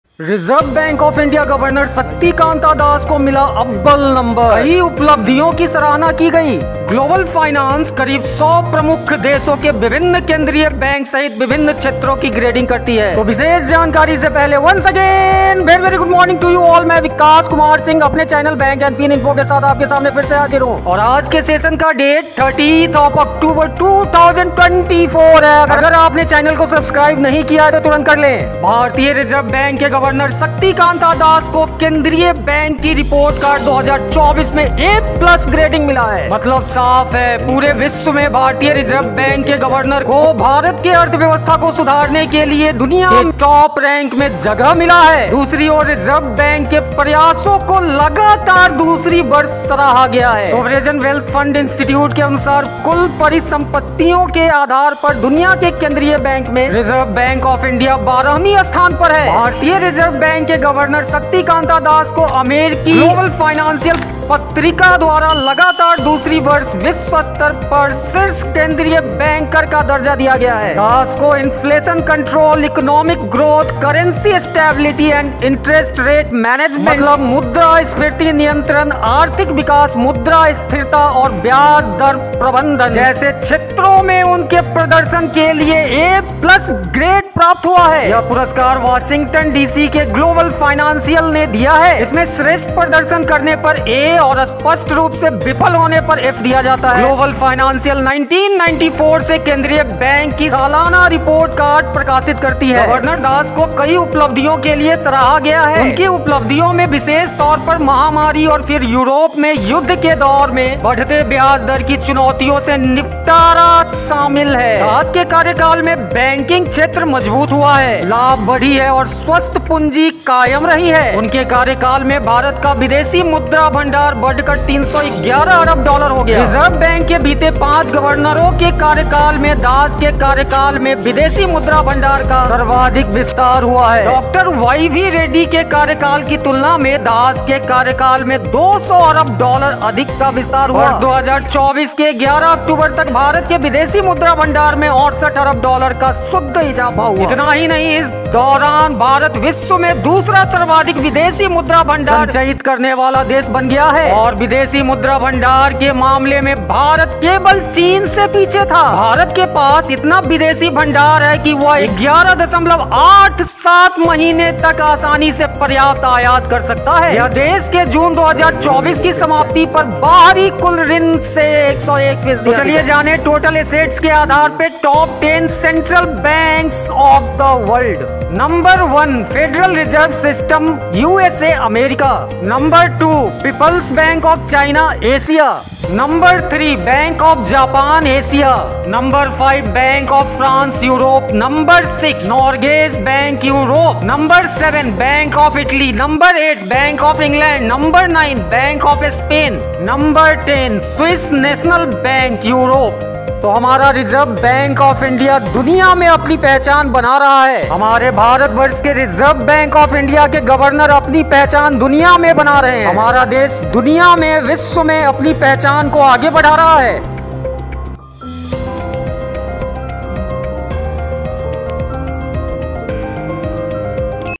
Globle Finance Ranking || RBI गवर्नर शक्तिकांत दास को मिला नंबर 1 का ख़िताब || RBI नंबर 12 पे
Globle Finance Ranking || RBI गवर्नर शक्तिकांत दास को मिला नंबर 1 का ख़िताब || RBI नंबर 12 पे || कई उपलब्धियों की सराहना

यह पुरस्कार वॉशिंगटन डी.सी. के ग्लोबल फाइनैंस ने दिया। इसमें प्रमुख क्षेत्रों जैसे महंगाई नियंत्रण, आर्थिक वृद्धि के लक्ष्य, हालिया स्थायित्व और ब्याज दर प्रबंधन की सफलता का आकलन कर ‘ए+’ से ‘एफ’ तक के ग्रेड दिए गए थे। इसमें सर्वश्रेष्ठ प्रदर्शन करने पर ‘ए’ और स्पष्ट रूप से विफल होने पर ‘एफ’ दिया जाता है।

ग्लोबल फाइनैंस 1994 से केंद्रीय बैंक की सालाना रिपोर्ट कार्ड प्रकाशित करती है। ग्लोबल फाइनैंस करीब 100 प्रमुख देशों, यूरोपियन यूनियन और कैरिबायाई व अफ्रीका के विभिन्न केंद्रीय बैंकों सहित विभिन्न क्षेत्रों की ग्रेडिंग करती है।

Link of Contents